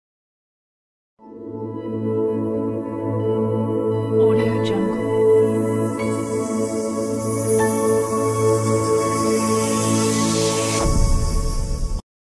After Effects Project Files - Clean Part Logo Intro - VideoHive 8454537
Lens flare created with optical flares, included is a pre-rendered version so no optical flares plugin is required Full HD 1920x1080 Easy to modify and customize background - simple for ... (logo-stings) Read more